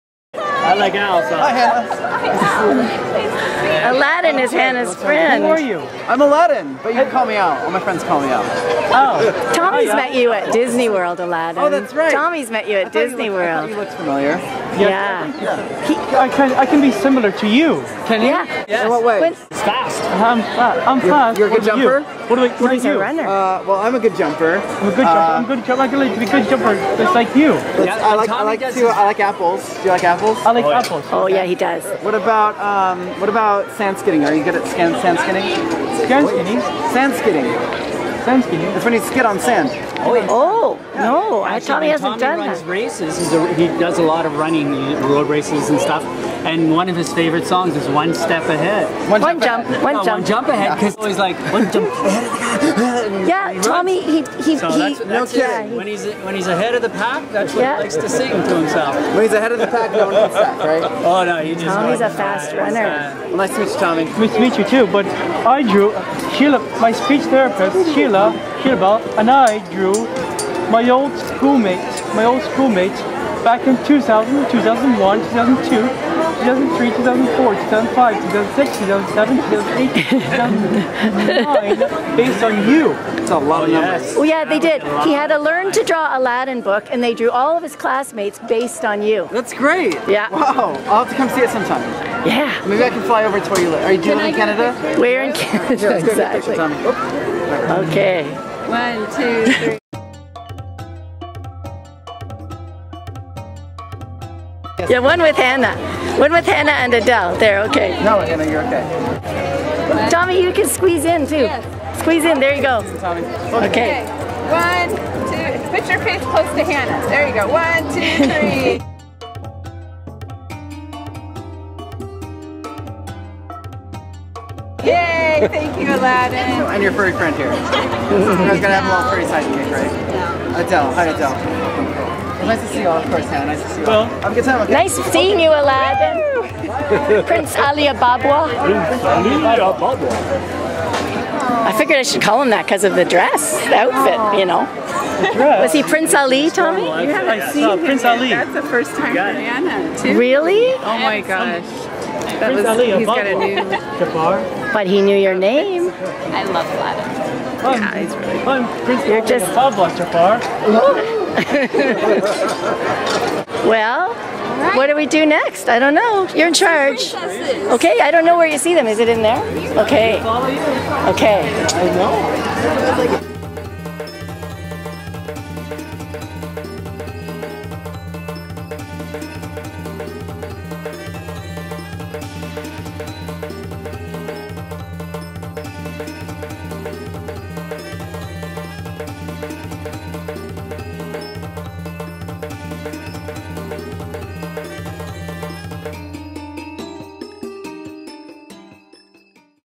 Tommy meets Prince Ali Ababwa Aladdin with Hannah at Disneyland
Prince Ali Ababwa ran up to greet Tommy and Hannah unexpectedly and magically!  Tommy loves Prince Ali, and asked him to be his guide runner one year for the Disney Wine and Dine Half Marathon.  Aladdin said he would have to use his magic carpet to keep up with Tommy!  Tommy even dressed like him for Halloween one year!  Search Tommy's channel for videos of Tommy dressed as Aladdin, and meeting Aladdin at Disney World!  It was awesome to see you again, Prince Ali!
Tommy is an Athlete with Autism who loves Disney!
Tommy's mantra is "I'm going to be the First Fastest Runner in the World!" Thank you for sharing his inspiring, joyous message ☺